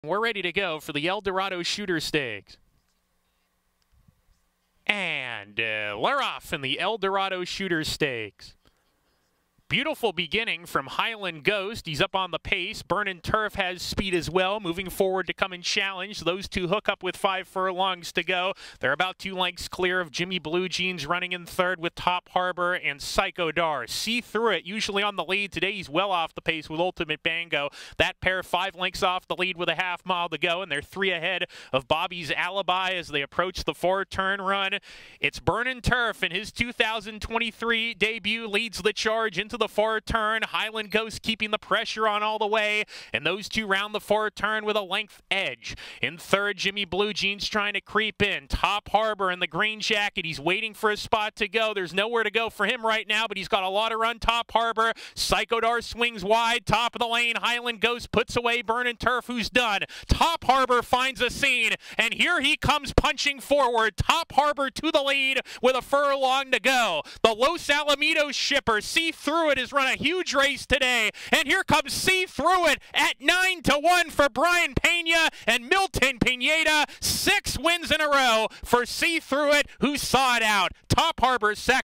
See Through It Wins the 2023 El Dorado Shooter Stakes at Golden Gate Fields: Race 8 on February 18.
Race 8 Replay on February 18, 2023 from Golden Gate Fields

Under jockey Brayan Pena, Los Alamitos shipper See Through It wins his sixth race in a row in the 2023 El Dorado Shooter Stakes at Golden Gate Fields.